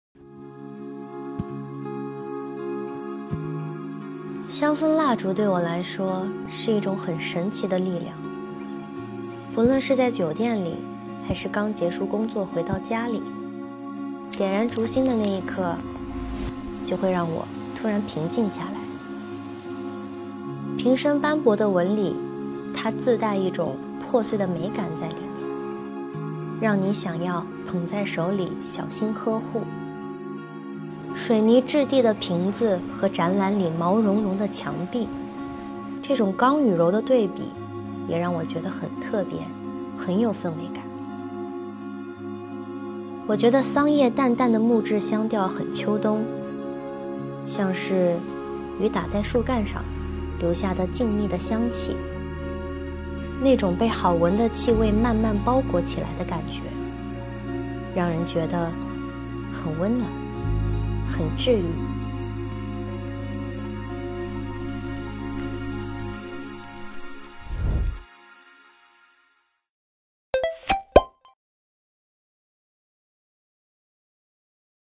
Zhou Jieqiong visits HAUS SHANGHAI Scented Candle Exhibition
221230 Jieqiong weibo update

“Come with me to the HAUS SHANGHAI. Archives Scented Candle Exhibition to explore the gentle aroma memories that permeate the space.”